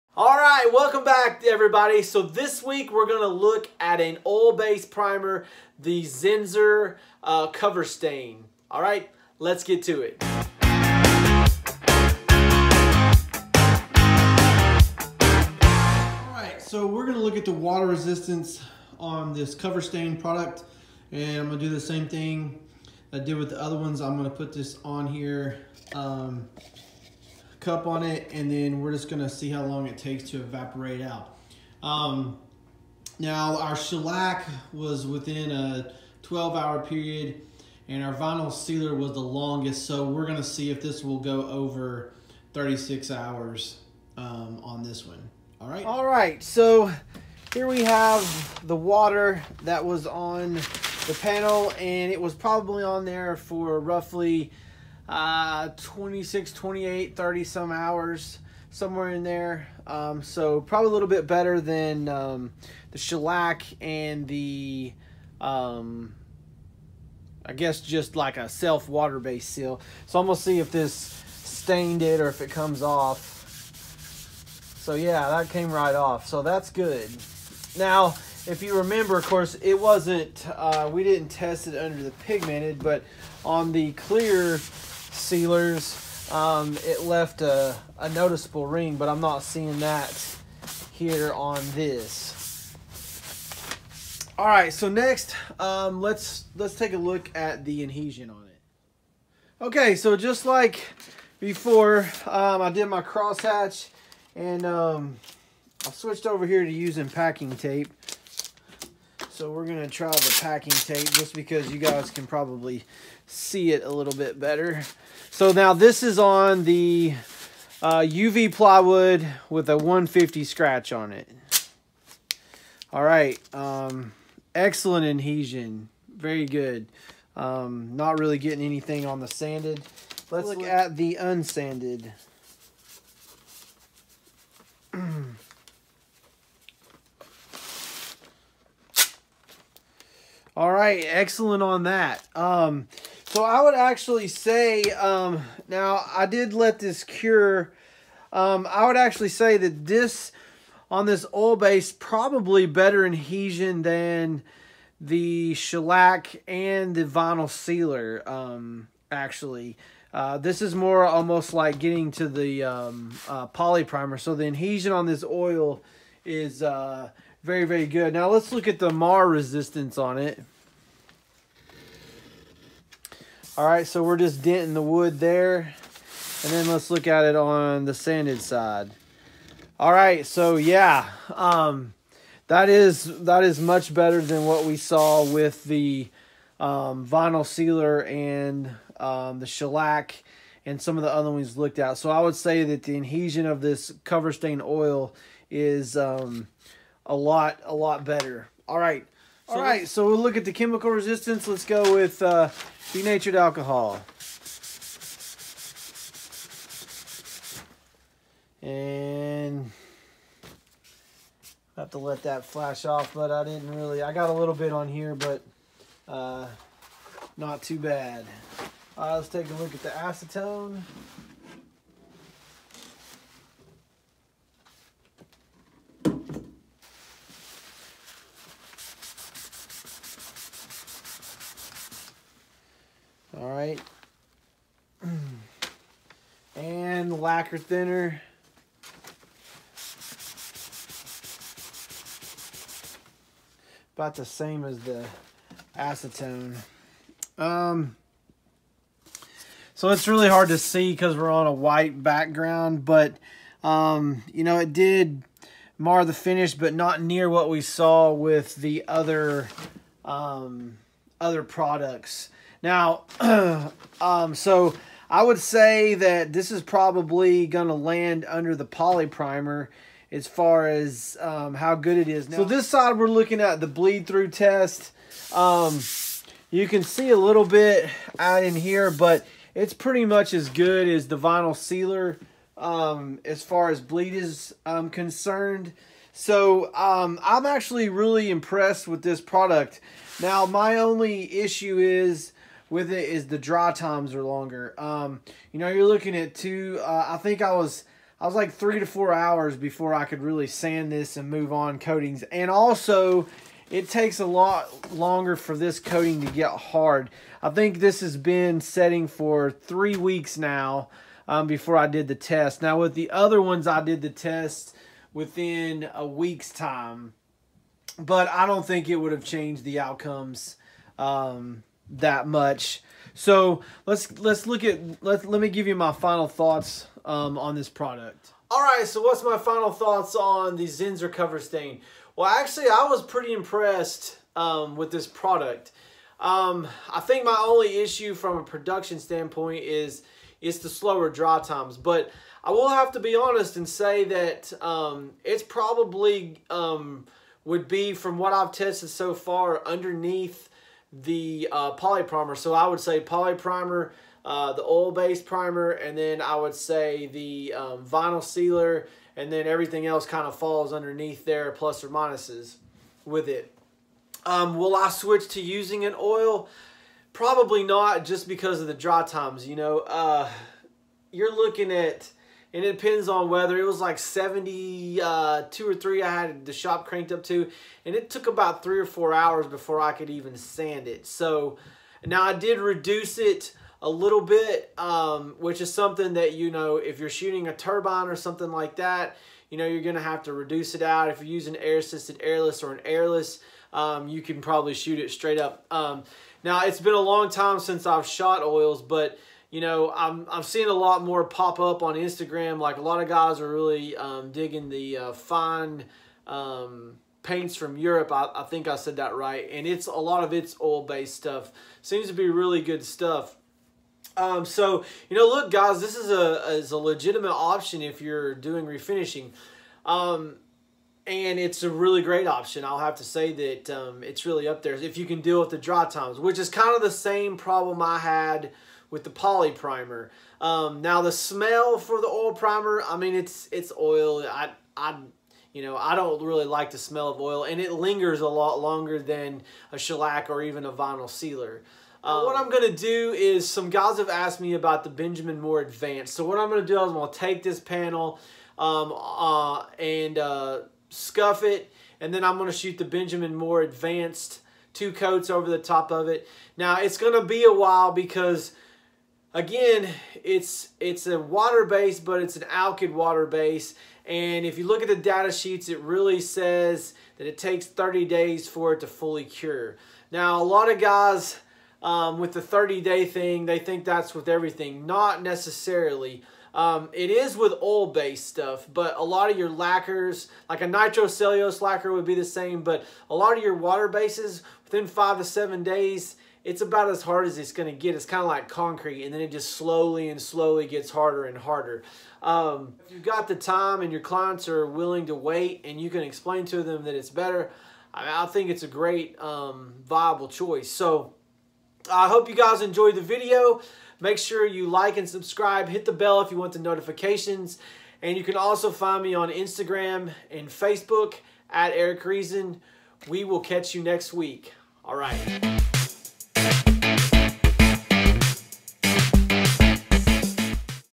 cover stain oil primer review
Due to poplar demand this  week we take a look at an oil based primer to see its adhesion, chemical, tannin blocking and water resistance qualities.

Technical data

One side of the panel a red dye was put on the surface to simulate tannin bleed and 1 coat of the zinsser cover stain was shot on the panel at 2-4 wet mils.  The other side of the panel was split in half with one half being sanded to 150 grit and the other was left unsanded.  Then the panel was sprayed at 2-4 wet mils with one coat of the primer. The panel was allowed to dry for 21 days before the test was preformed